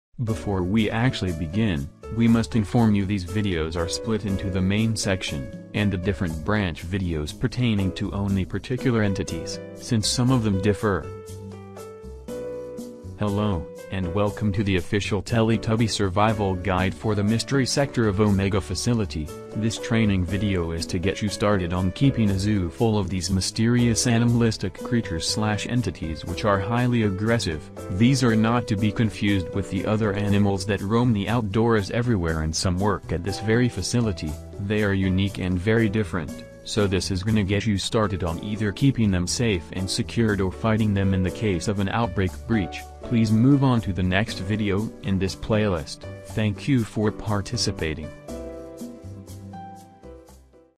Omega Facility Training Video 1: Introduction
""Inspired"" by various EAS Scenario / VHS Tape vidyas, except I chose a more comedic and less effort version, still describing stuff and in this case it's just basic etiquette on HOW TO HANDLE Evil Entities